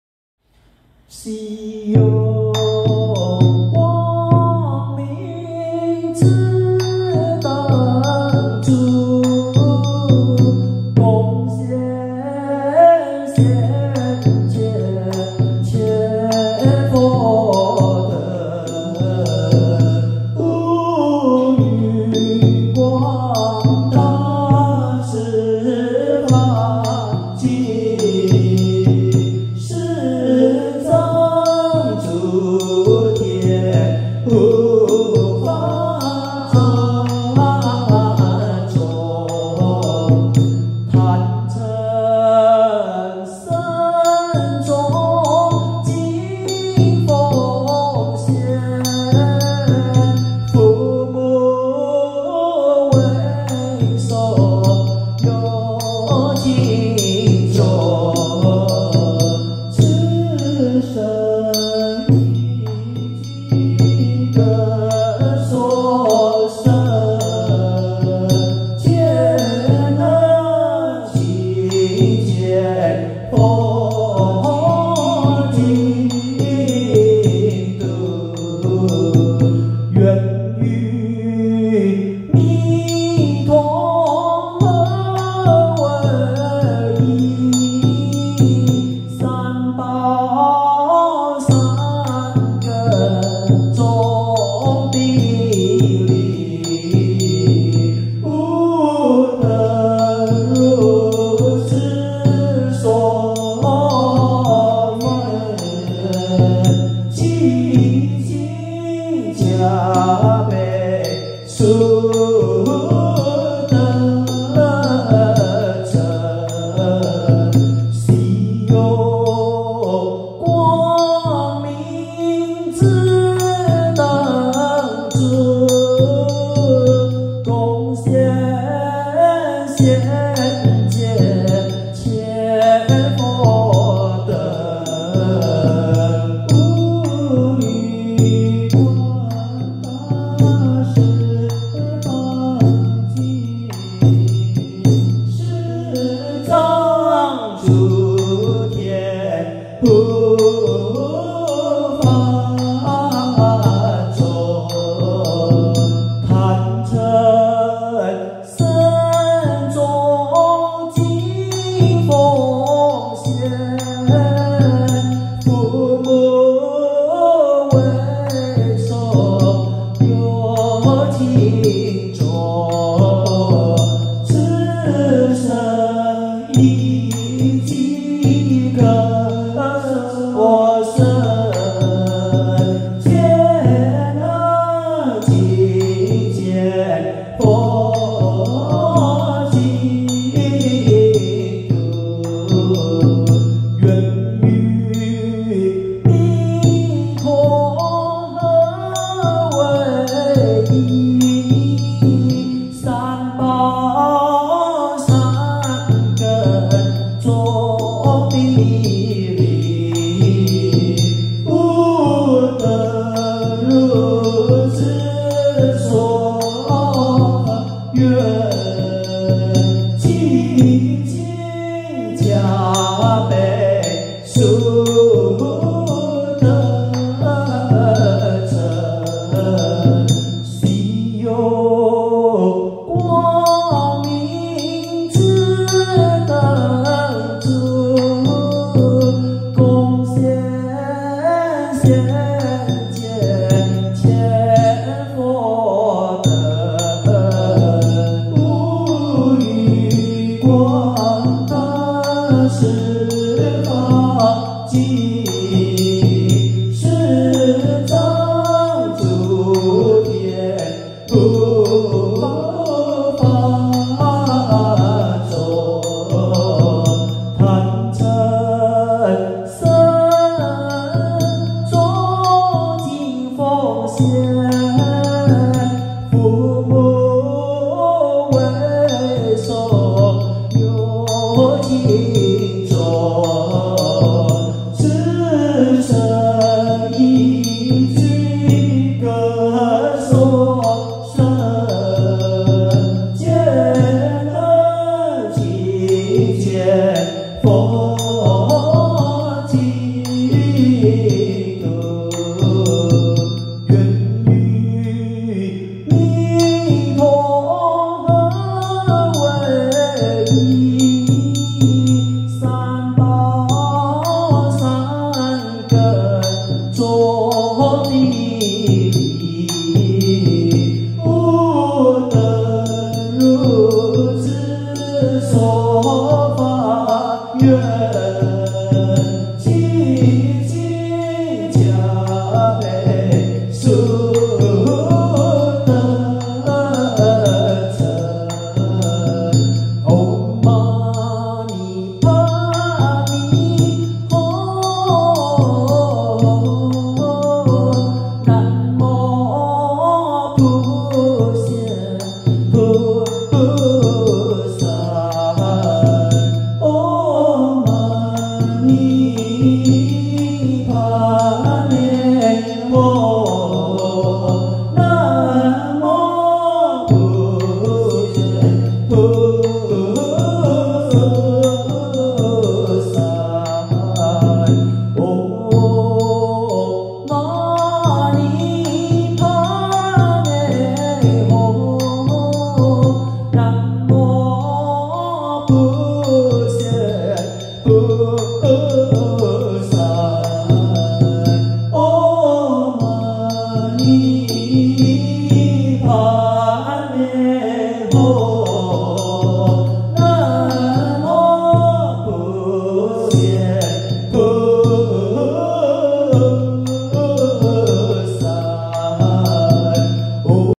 VĂN KHẤN NGUYỆN THẮP NẾN
Ánh sáng hiếm có của nến này
Dâng cúng chư Phật trong Hiền Kiếp
Cùng khắp hà sa mười phương cõi
Sư Trưởng, Chư Thiên, chư Hộ Pháp
Thánh Chúng chứng đàn đồng dâng cúng
Cha Mẹ cùng các chúng hữu tình
Sanh đây và sanh những nơi khác
Đều được nhìn thấy cõi nước Phật
Nguyện cùng Di Đà hợp làm một
Oai lực Tam Bảo cùng Thánh Chúng
Sở nguyện chúng con là như thế
Cầu xin gia bị chóng đạt thành.
Light Offering
With this radiant light, wondrous and precious
We reverently offer to the Thousand Buddhas of Worthy Kalpas thus
To all the vast and great realms in the ten directions without bound
To Honored Teachers and Dharma Protectors all around
To all the Sagely Ones within the pure Maṇḍalas respectfully we offer.
To our foremost parents along with sentient beings without number
In this lifetime and all places of rebirth alike
May all behold the Buddhas' Pure Lands in sight
Vowing to always be by Amitābha Buddha's side
With the Triple Jewel's power of Truth insights
Along with Great Beings of the Three Roots erudite
This prayer and vow we hereby reverently give rise
We respectfully ask for your blessings so they can be quickly realized